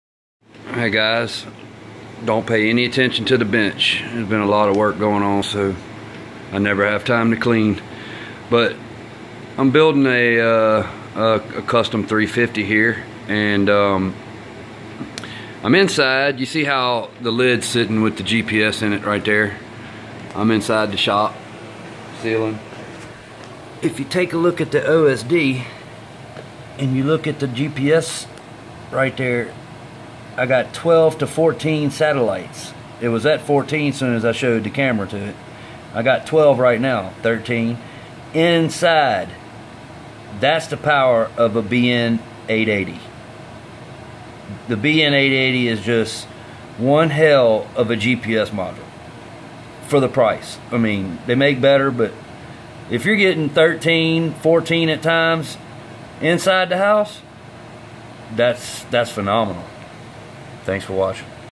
The Power And Wonder Of A BN-880
When I was working on this quad in the shop, I noticed on the OSD that I had 12-14 sats. INSIDE! That's the power and beauty of the BN-880. It's one of the easiest things to install on the X350 Pro. It's a direct swap out and is the very same formfactor and manufacture of the stock unit. The stock GPS does not have a built in compass but the BN-880 does and it isn't used because it's a whole lot better to continue to use the compass on the leg. It is the very same compass chip. The stock GPS cable only has the pins for power and the TX/RX for the GPS alone. The other 2 pins for the compass are open.